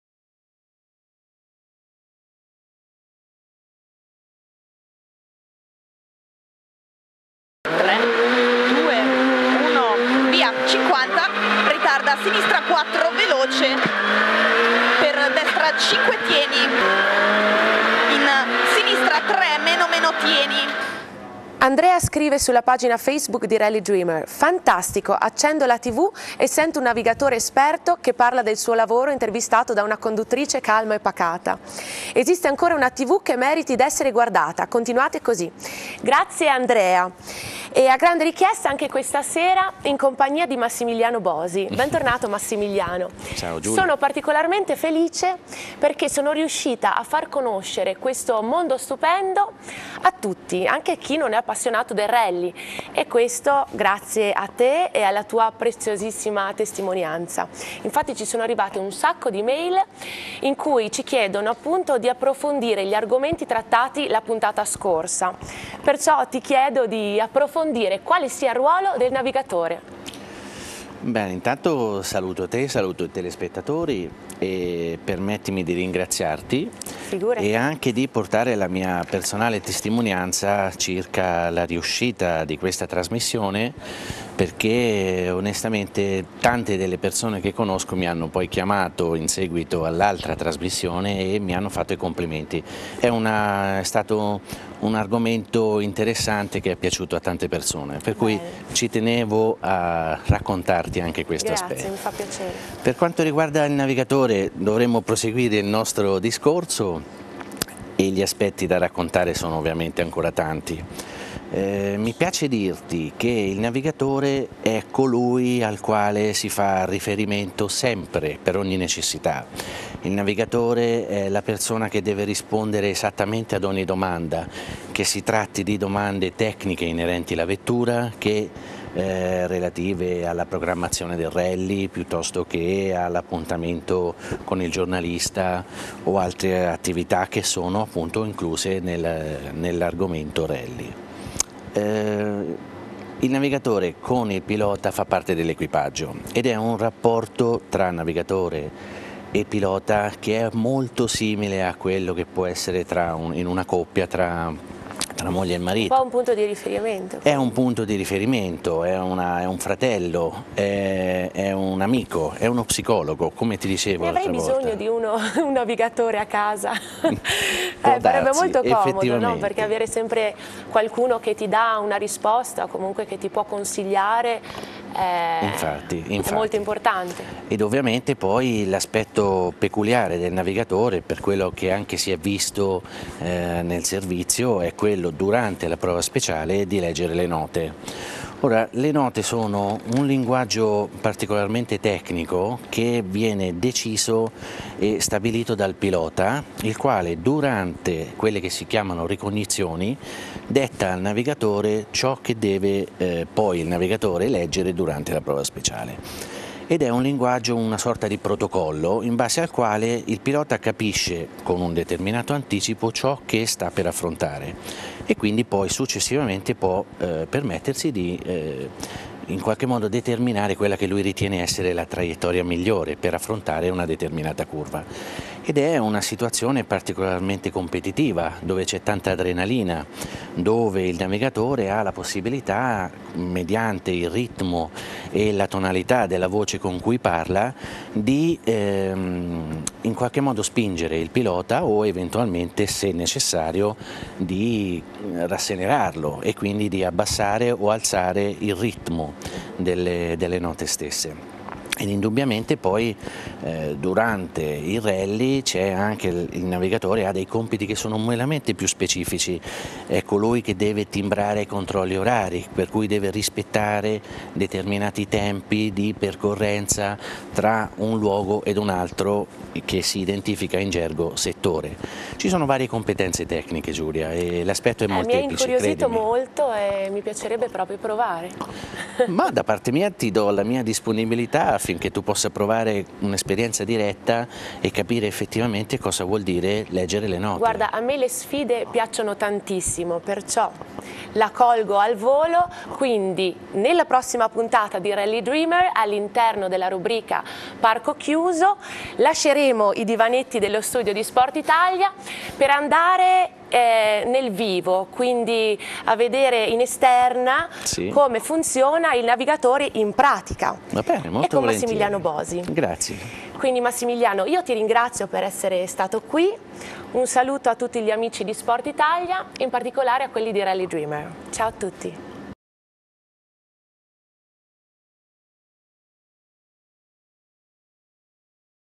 Rally dreamer quarta puntata: Rubrica Parco chiuso
Anche in questa puntata approfondiremo il ruolo del Navigatore con Massimiliano Bosi